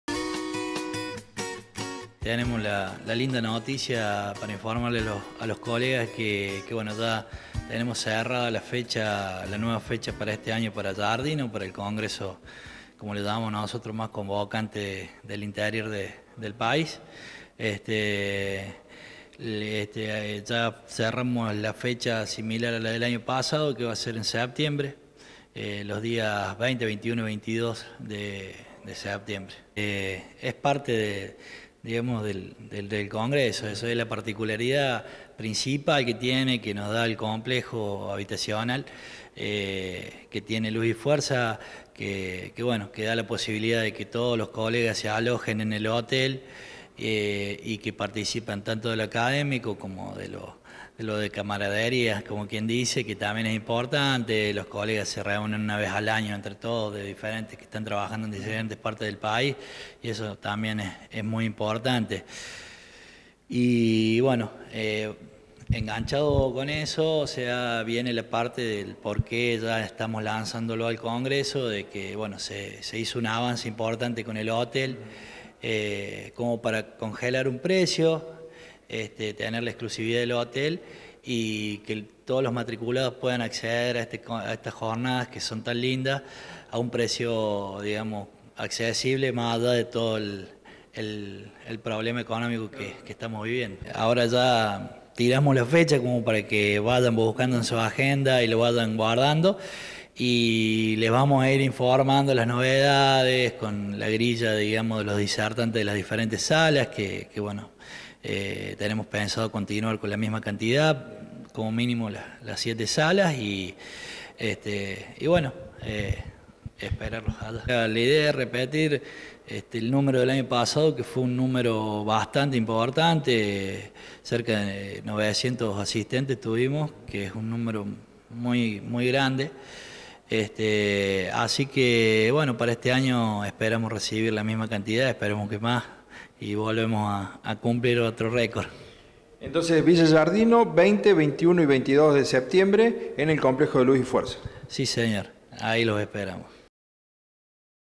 Las Jornadas Veterinarias ya tienen fecha
El Colegio Médico Veterinario ya fijó fecha a su principal Jornada Anual en Villa Giardino.   Fuente: Campo Directo